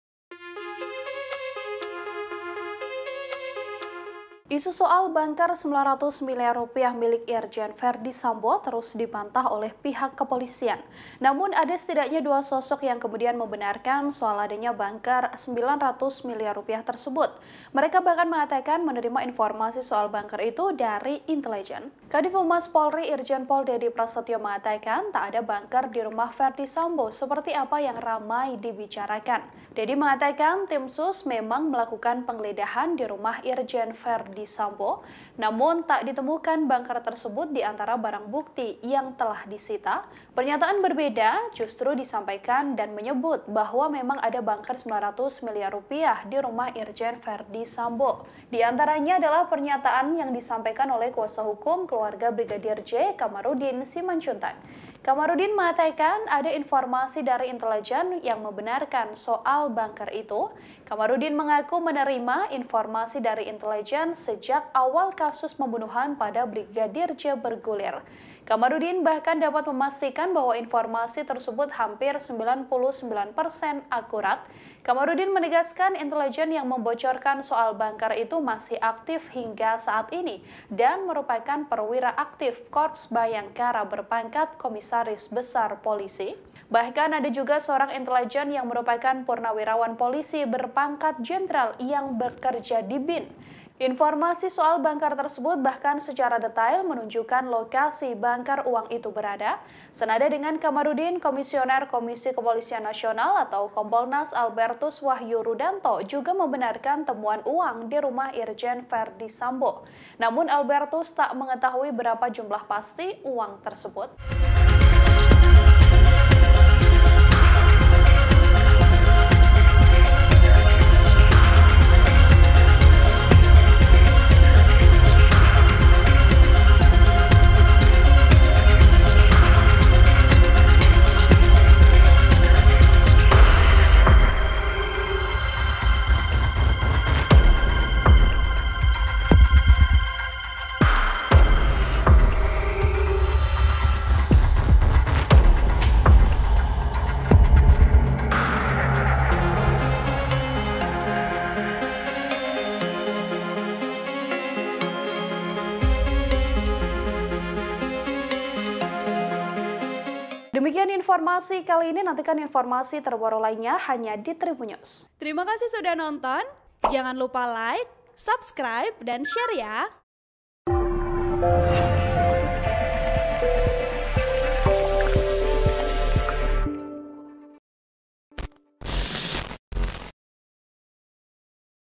Ada Kombes & Jenderal BIN Benarkan Bunker Rp 900 M di Rumah Ferdy Sambo saat Mabes Beri Bantahan
TRIBUN-VIDEO.COM - Isu soal bunker Rp 900 miliar milik Irjen Ferdy Sambo terus dibantah oleh pihak kepolisian.

Namun ada setidaknya dua sosok yang kemudian membenarkan soal adanya bunker Rp 900 miliar itu.

Mereka bahkan mengatakan, menerima informasi soal bunker tersebut dari intelejen.